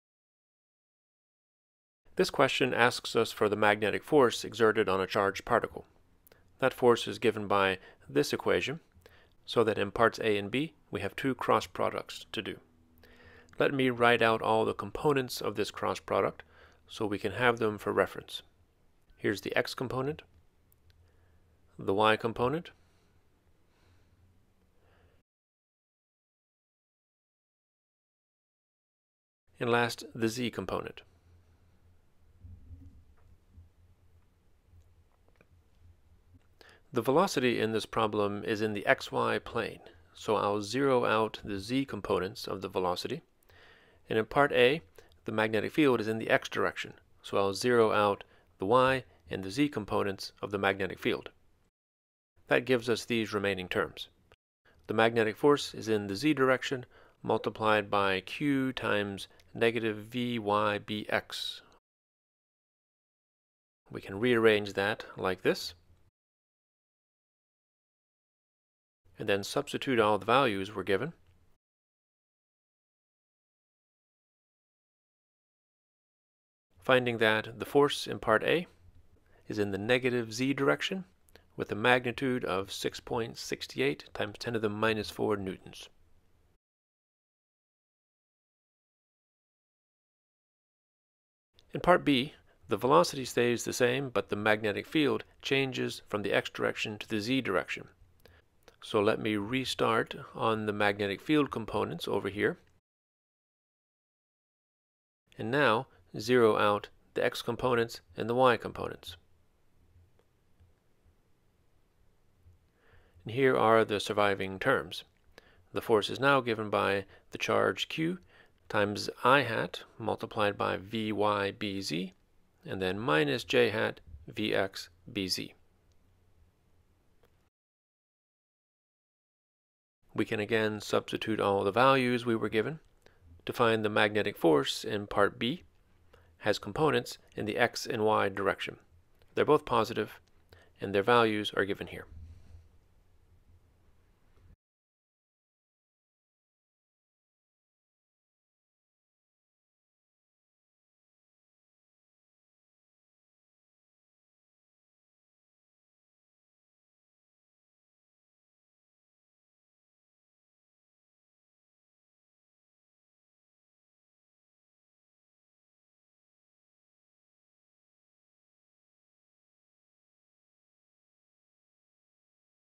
E&M: Magnetic Force. Level 1, Example 1
A particle of charge q = –1.24 × 10–8 C moves with velocity (an equation). What force is exerted on the particle by a magnetic field of (a) (an equation) and (b) (an equation) ?